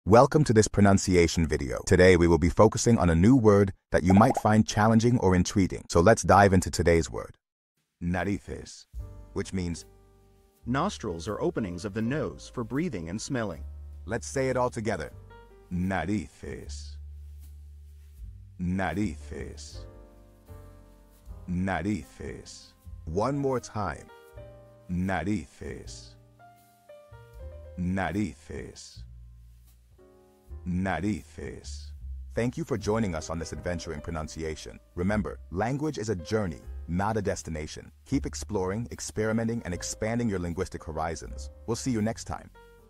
How to pronounce Narices
Master the Pronunciation of 'Narices which means noses' - which means : Nostrils or Openings of the Nose for Breathing and Smelling. 📝 with @PronunciationSpanish 🎓🗣️ - [Your Guide to Spanish]

Learn to pronounce 'Narices' perfectly in Spanish with our easy and engaging tutorial by @PronunciationSpanish! 📖📽️ In this comprehensive video, we provide you with native speaker examples 🗣️👂 and phonetic breakdowns 🎵 to ensure you get the pronunciation just right.

How to say Nose in Spanish